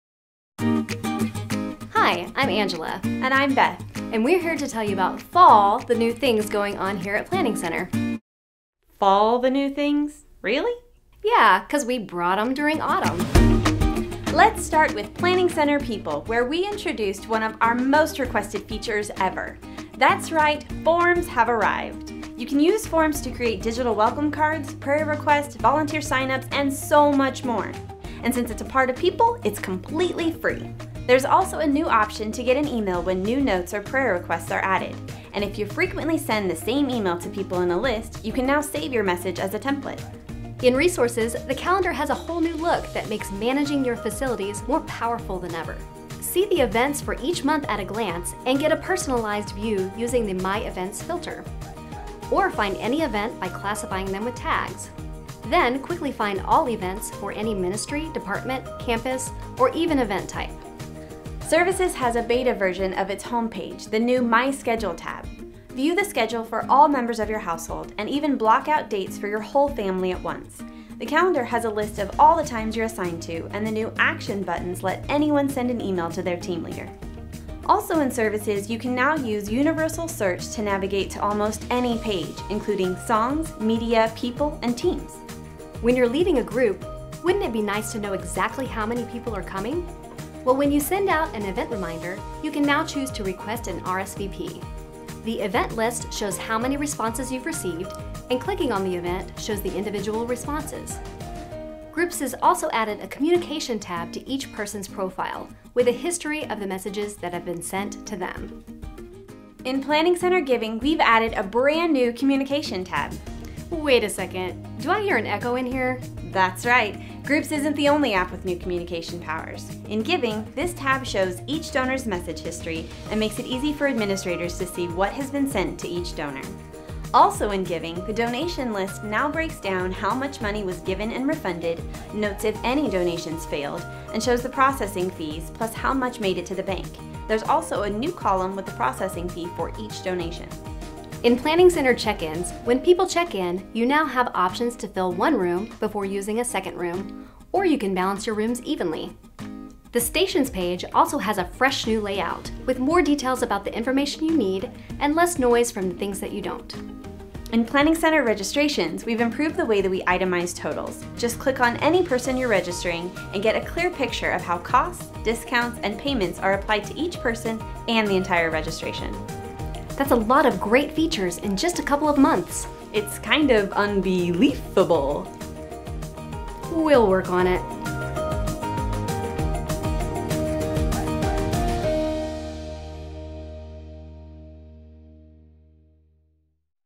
Planning Center Fall Product Update 2018
Over the last few months, our team has been hard at work on new features across our suite! Watch our fall product update to learn how these new Planning Center tools can support your ministry!